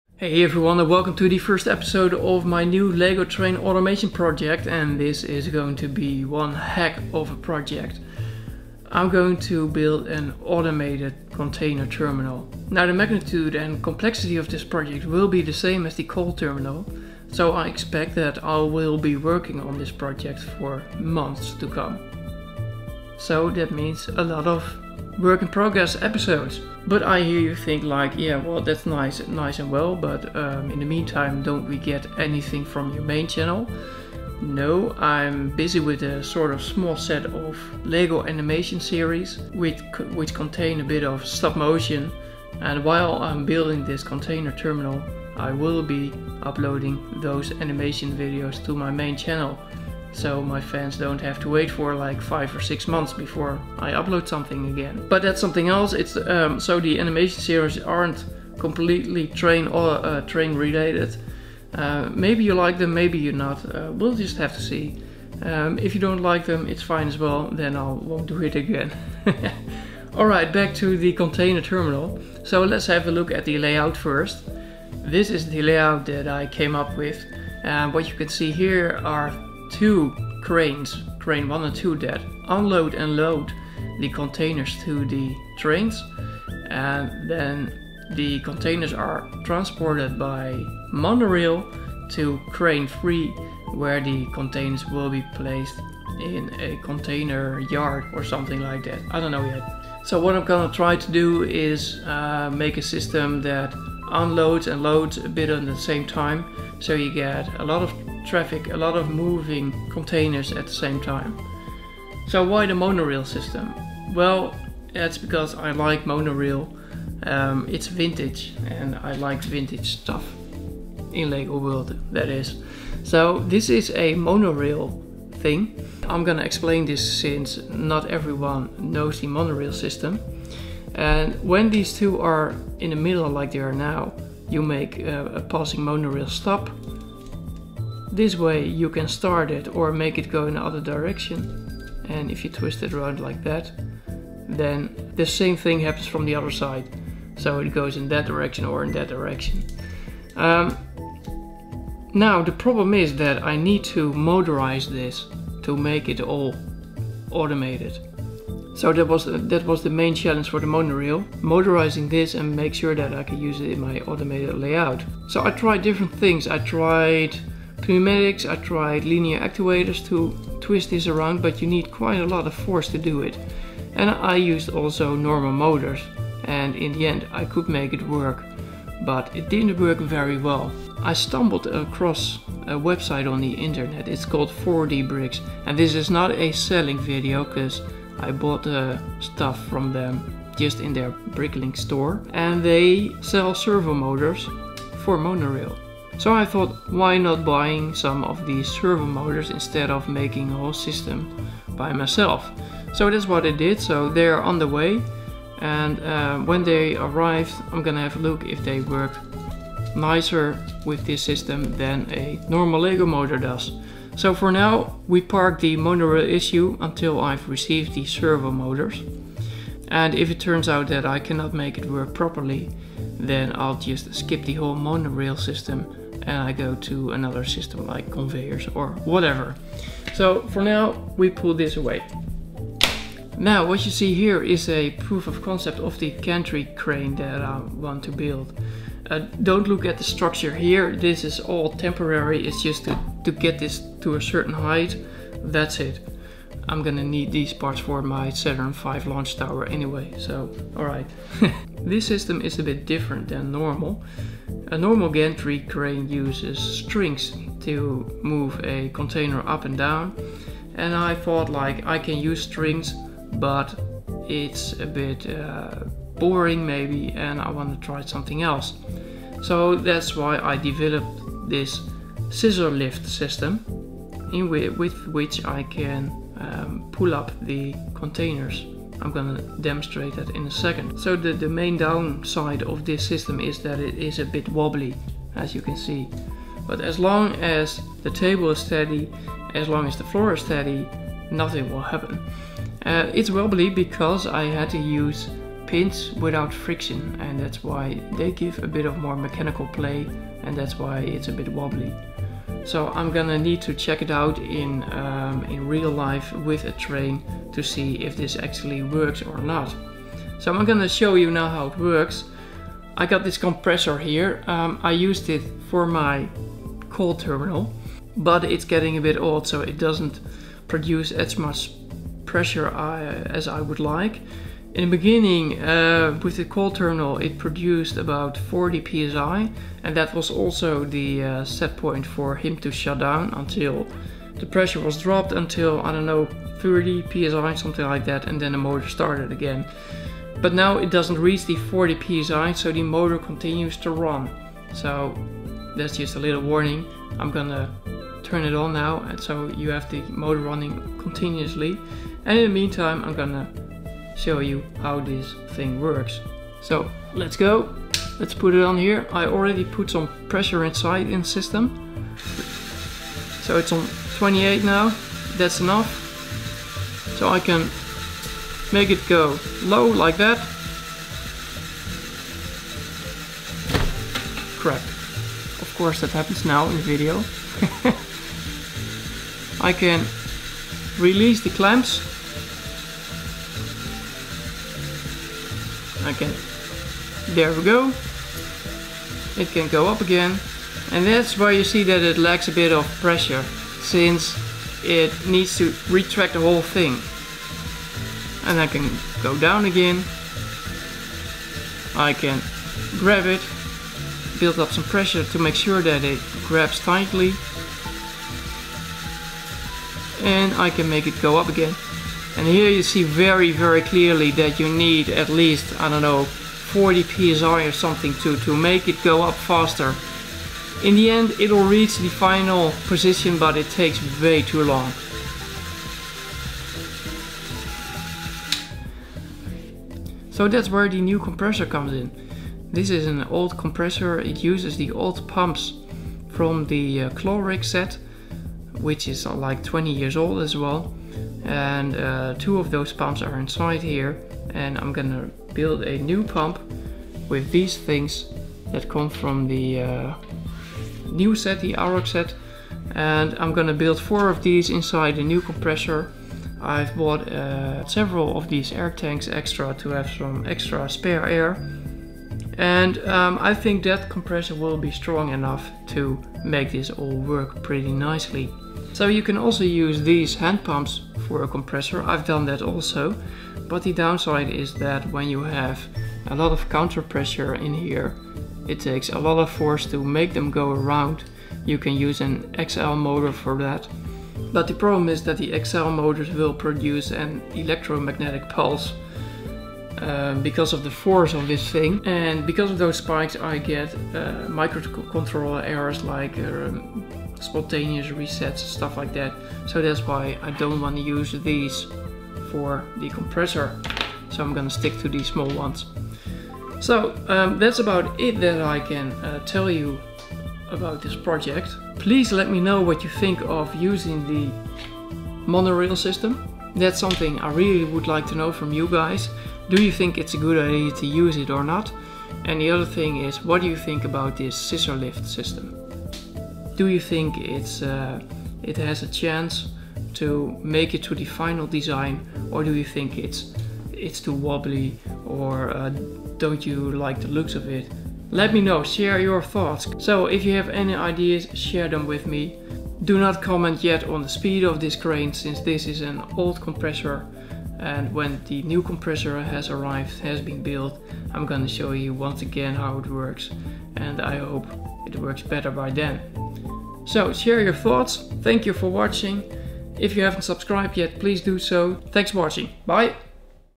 Lego train container terminal WIP E01: layout and proof of concept
Lego train container terminal (fully automated by Arduino) Work In Progress series episode 1!

In this episode I explain the layout including Lego monorail and a proof of concept of a gantry crane with a scissor lift construction.